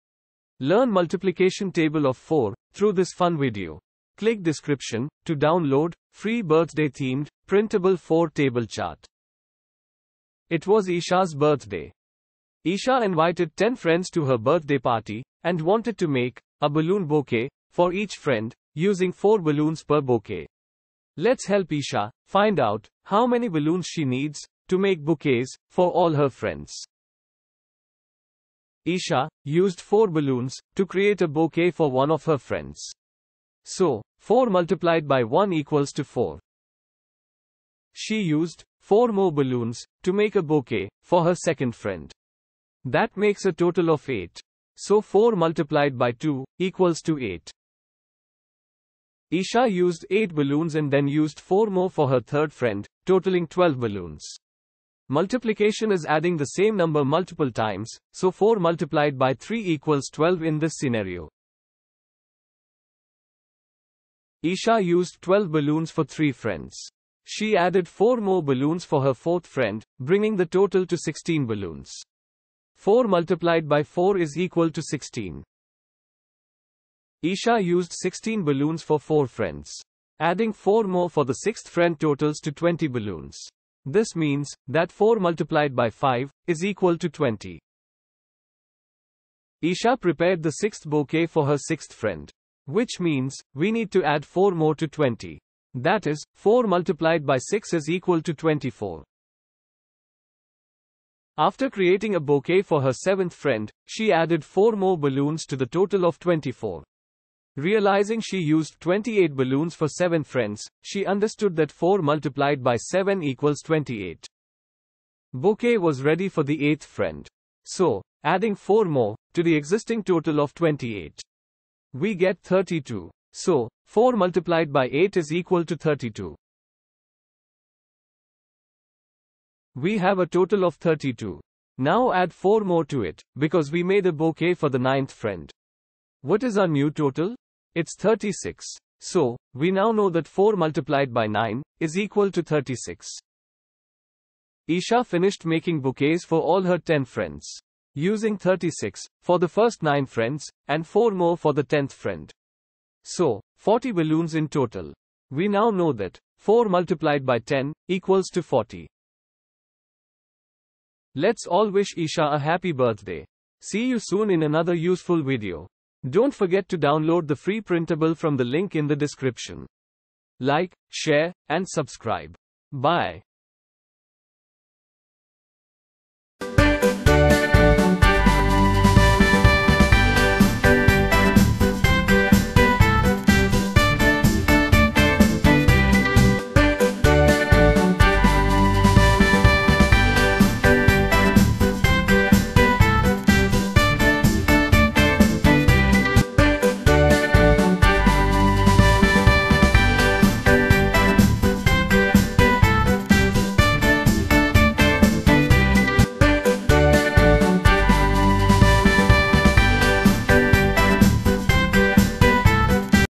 MultiplicationTable 4 | FREE PRINTABLE MultiplicationTable From Link In Description| PlayLearnCraft
Immerse yourself in a 🎉🎈fun and vibrant birthday-themed🎂🎂🎂 math video to master the❌ "4 times table"❌. Join the birthday girl in creating balloon 🎈🎈bouquets for her friends while learning multiplication in an engaging way.